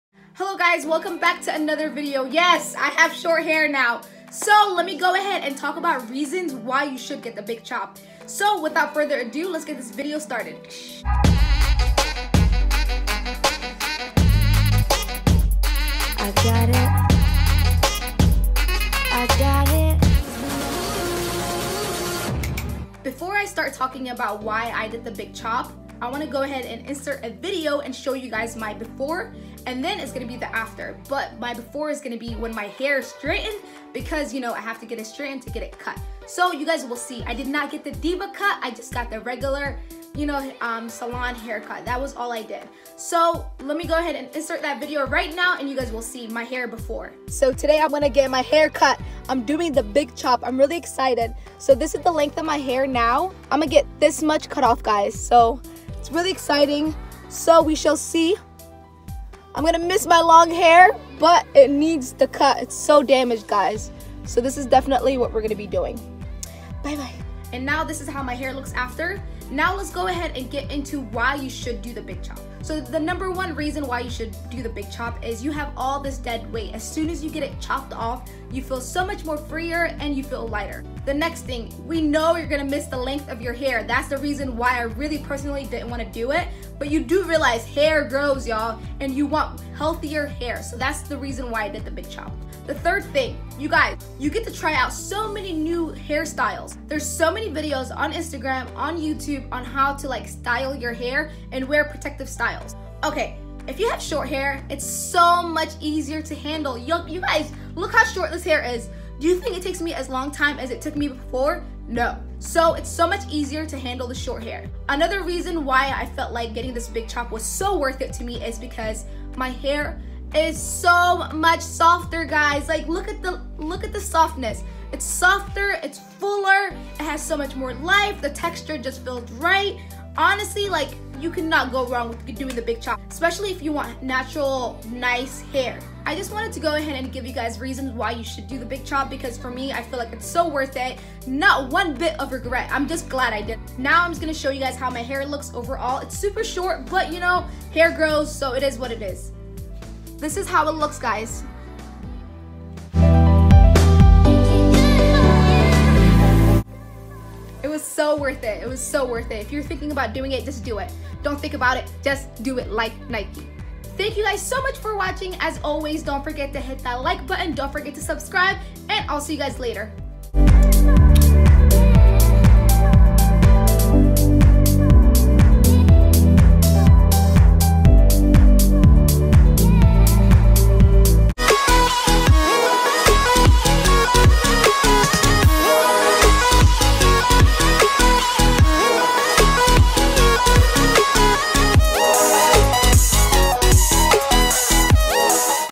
THE BIG CHOP| CUTTING MY NATURAL HAIR
Hello lovely people, guess what I did ?! Yes I definitely did it and in this video we talk about why I got the big chop.

Super random, if you don’t know what Ebates is be sure to check it out. Cash back for just SHOPPING ONLINE!